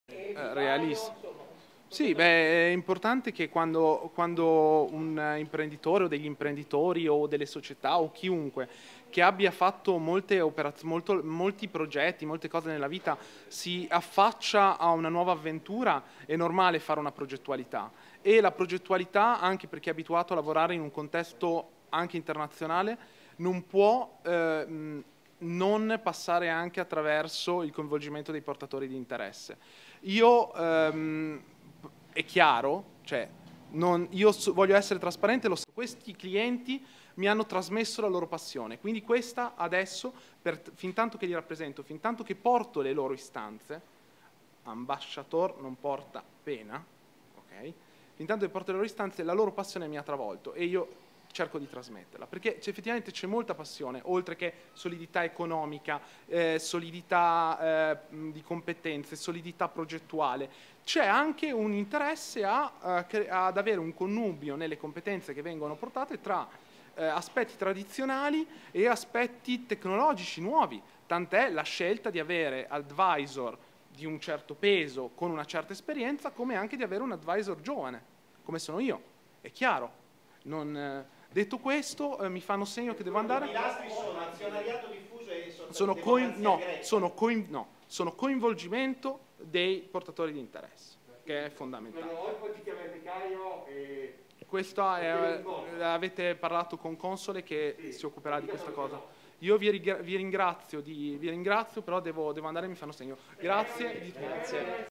Giuseppe Pipicella: «Questi clienti mi hanno trasmesso la passione per il Toro»
Intervista a Giuseppe Pipicella, advisor del Comitato Taurinorum, intervenuto al termine della conferenza stampa per spiegare il progetto relativo al Torino